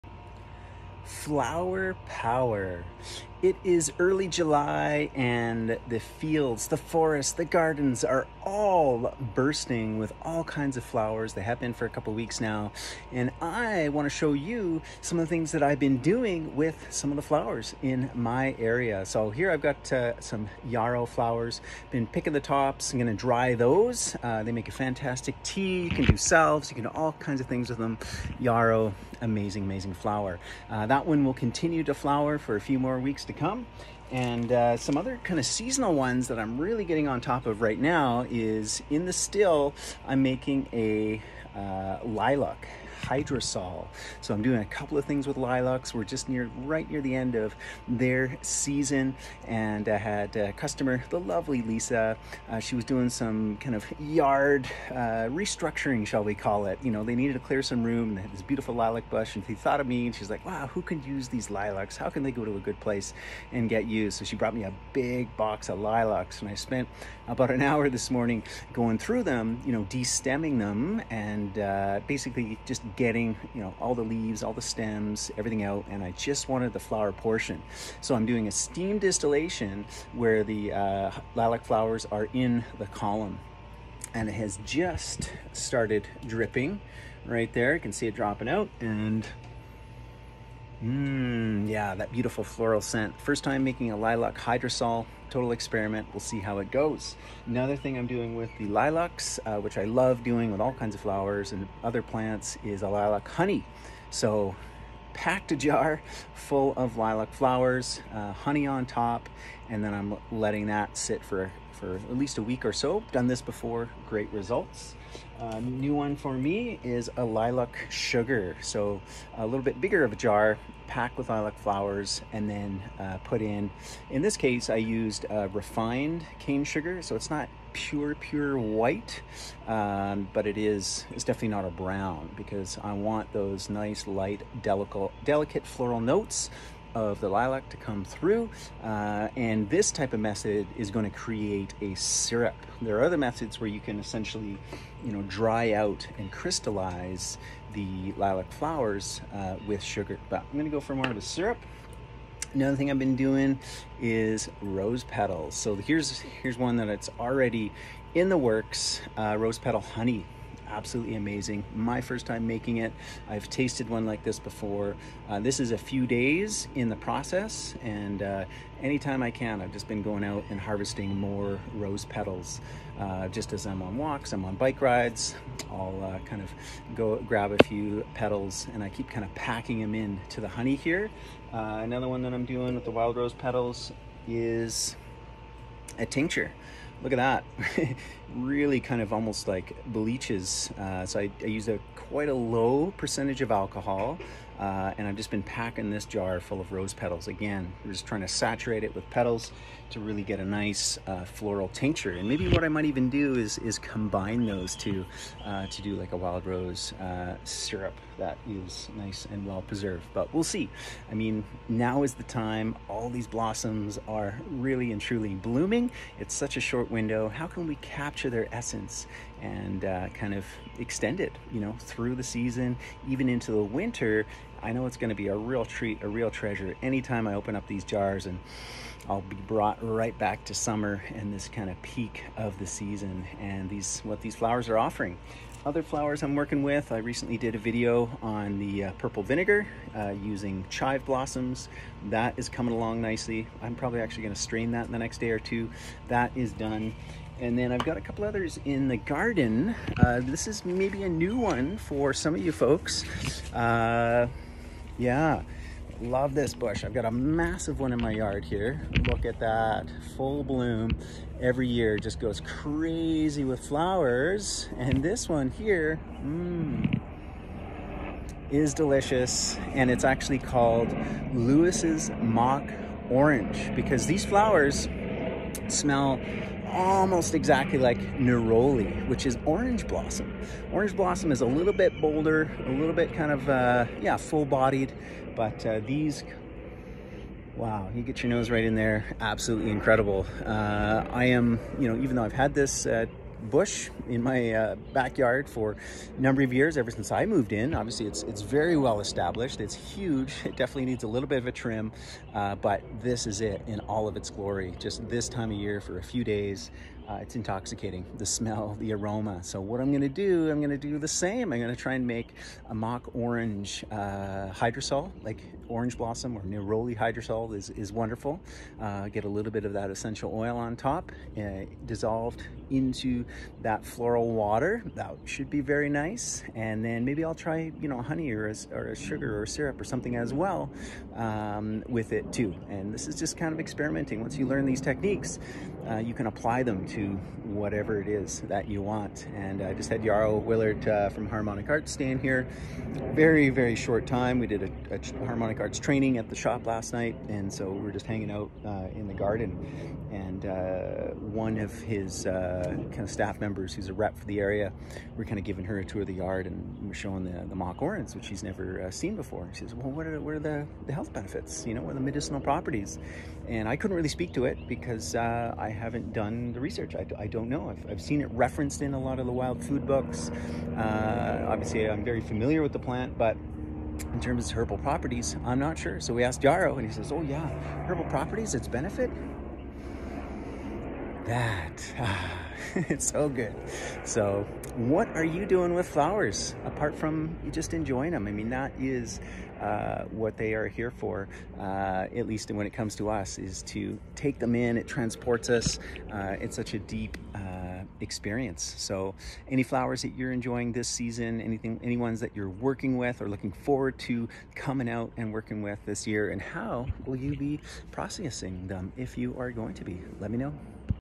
Flower Power: Various Ways to Capture & Store the Beauty of Summer Blooms
Tis the season where there are so many beautiful and wonderfully aromatic flowers blooming right now
Here’s what I’m working with and doing with them to appreciate and capture their essence